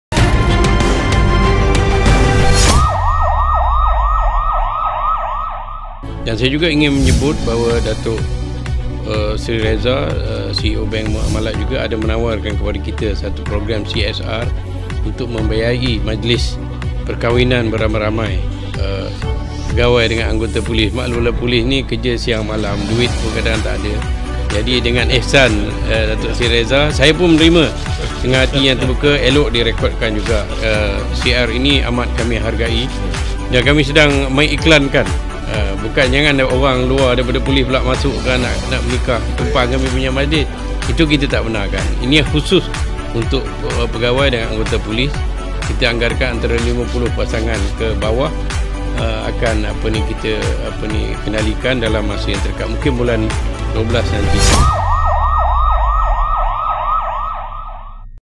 MKL Crimedesk | "Anggota PDRM ditaja perkahwinan" - IGP
BANK MUAMALAT MALAYSIA BERHAD ,BANK MUAMALAT BAKAL MEMBIAYAI MAJLIS PERKAHWINAN KIRA-KIRA 50 PEGAWAI DAN ANGGOTA POLIS, HUJUNG TAHUN INI.

KETUA POLIS NEGARA, TAN SRI MOHAMAD FUZI HARUN BERKATA, PIHAKNYA MEMBUKA PELUANG ITU KEPADA MANA-MANA PEGAWAI DAN ANGGOTA YANG MASIH BELUM PERNAH BERKAHWIN.

KATANYA KEPADA PEMBERITA PADA SIDANG MEDIA SELEPAS MENYAKSIKAN MAJLIS MENANDATANGANI MEMORANDUM PERSEFAHAMAN ANTARA YAYASAN PENGAMAN DAN BANK MUAMALAT, SIANG TADI.

MENGULAS LANJUT, MOHAMAD FUZI BERKATA SOAL LOKASI TIDAK MENJADI MASALAH KERANA PIHAKNYA MEMPUNYAI BEBERAPA CADANGAN YANG SESUAI.

PDRN SUDAH PUN MEMILIH BEBERAPA TEMPAT, MUNGKIN IBU PEJABAT POLIS KONTINJEN SELANGOR, PULAPOL ,PUSAT LATIHAN POLIS ATAU MAKTAB PDRM.

JELASNYA, TEMPAT TIADA MASALAH CUMA MAHU MENDAPATKAN PENGANTIN DULU. MAJLIS INI DIJANGKA BERLANGSUNG DISEMBER INI.